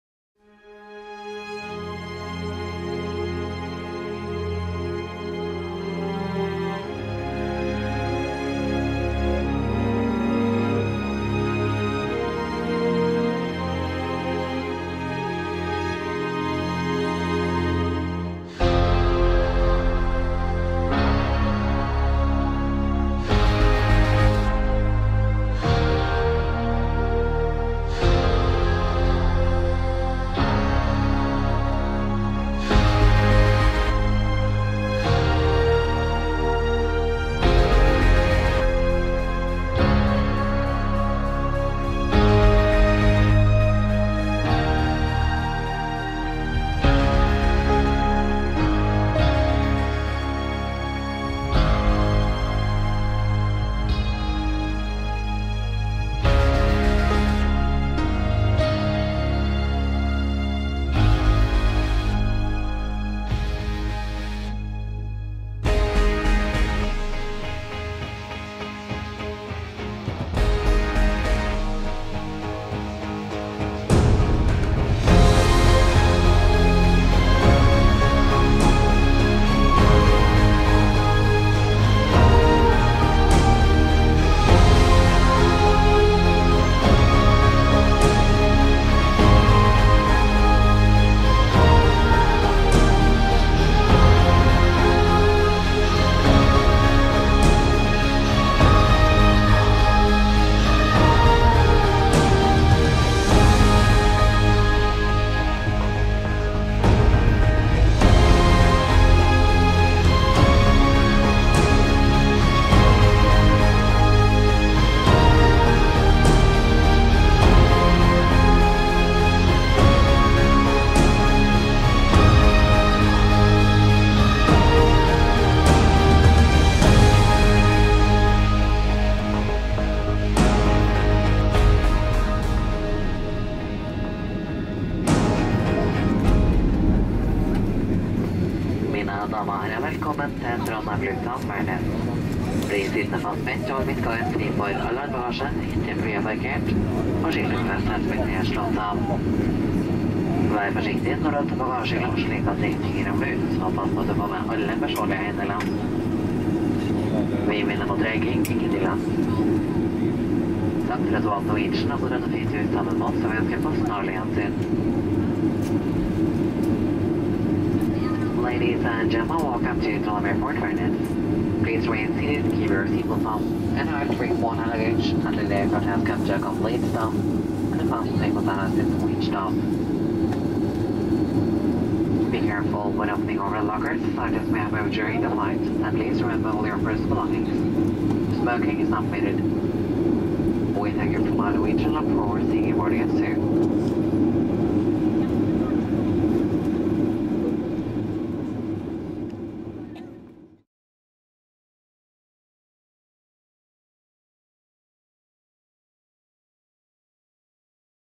DY764 OSL-TRD Norwegian
Tror det var LN-NID vi satt på denne gangen. Nedstigning via Selbu og inn mot bane 27 på Værnes i fra øst.